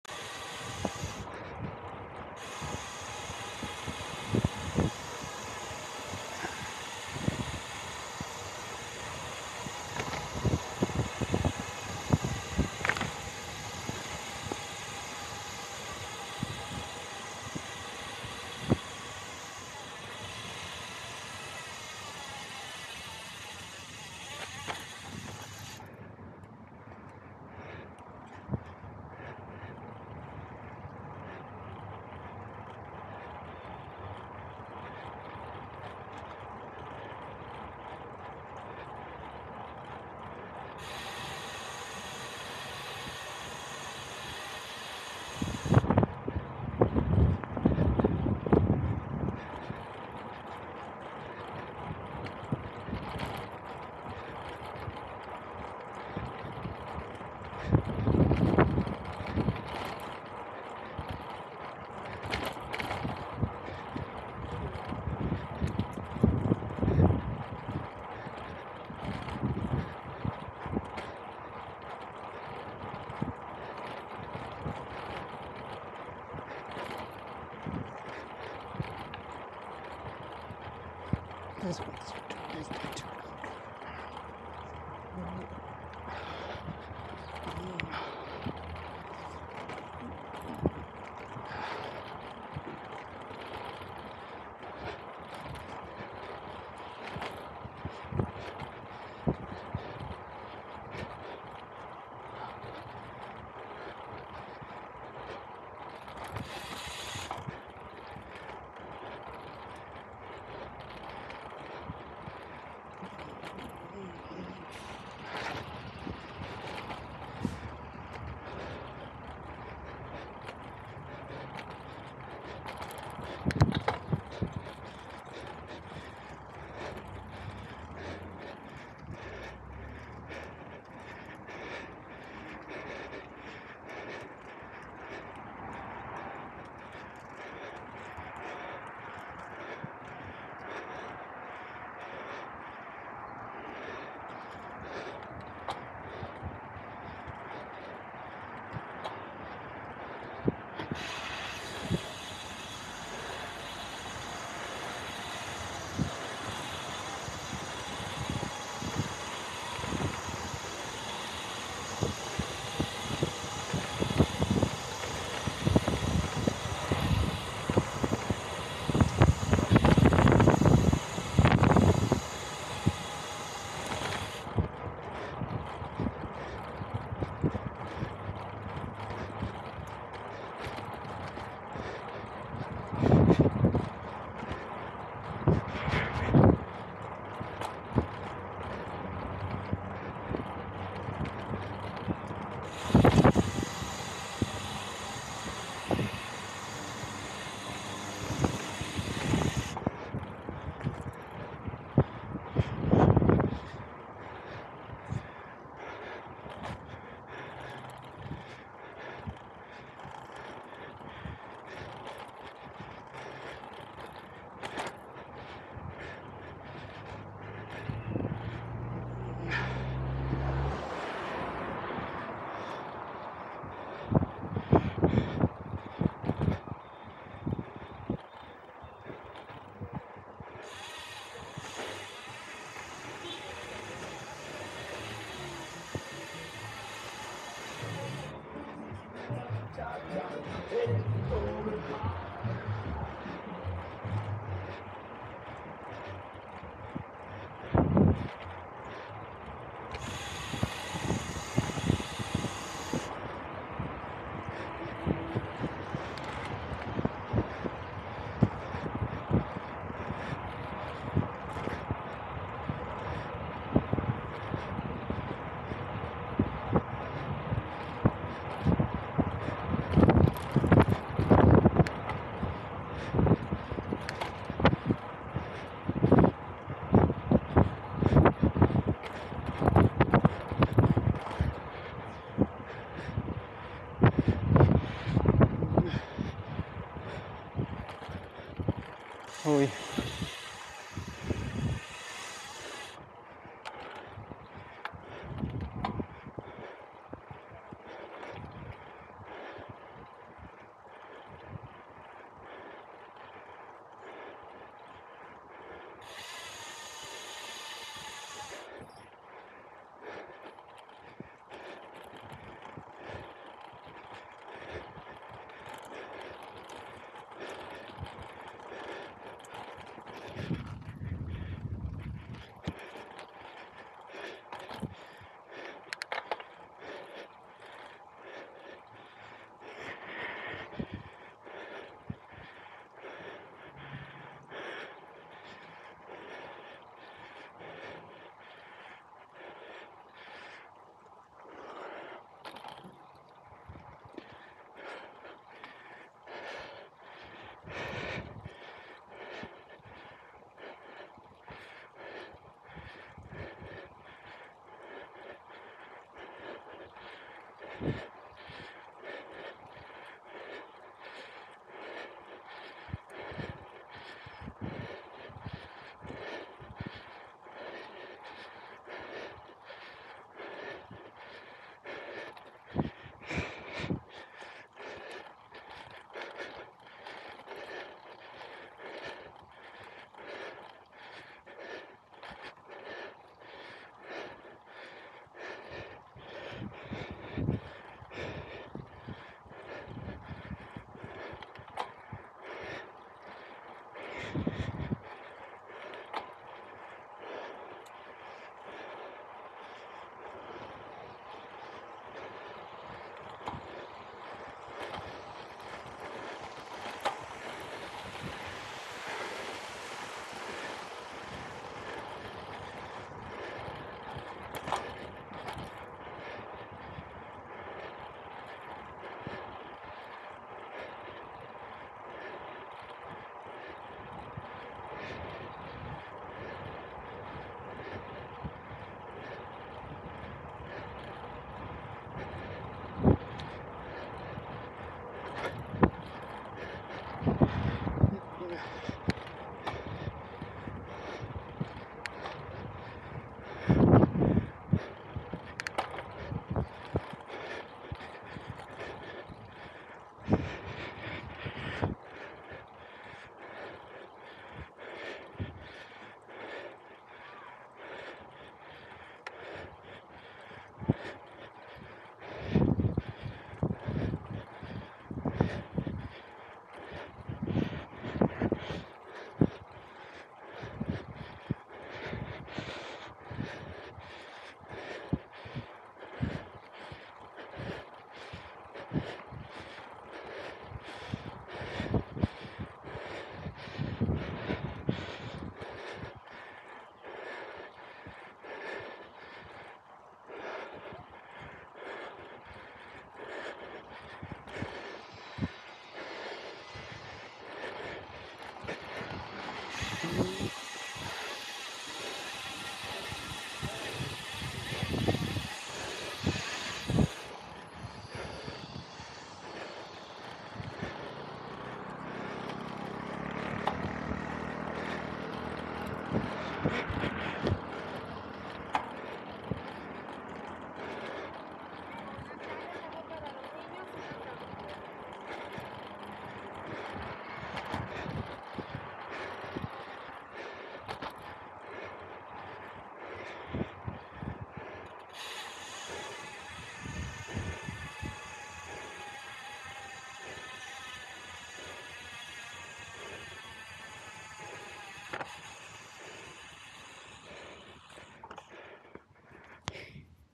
Seunda sección de Chapultepec
CDMX, México Con una Insta360 a la que se le junto humedad/condensación al lente...